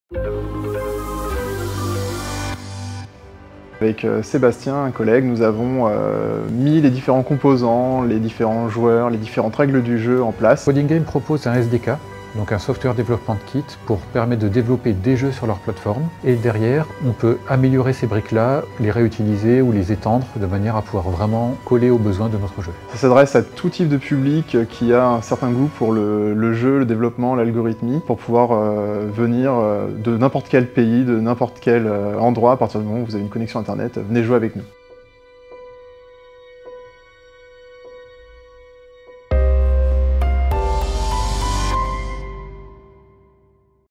[GREEN CIRCLE] #5 avec Sebastien et Jérôme les créateurs du jeu
Les développeurs du monde entier peuvent jouer sur Green Circle, un jeu au concept inédit développé par Société Générale en collaboration avec CodinGame.

Le principe est simple : coder de la manière la plus vertueuse possible.

Ce jeu de code est conçu pour sensibiliser les développeurs au Green IT. Très engagé sur le sujet, Société Générale a signé, dans la lignée de ses engagements RSE, la Charte du Numérique Responsable en 2019, et lancé l’année dernière une série de Masterclasses, créées par ses experts, pour partager ses convictions sur les différents aspects du Green IT.

La gamification est un moyen original pour sensibiliser un très grand nombre de développeurs.

Concrètement, les participants sont dans la peau d’un développeur agile, ou « agile master », dont l’objectif est de coder des applications de la manière la plus vertueuse possible.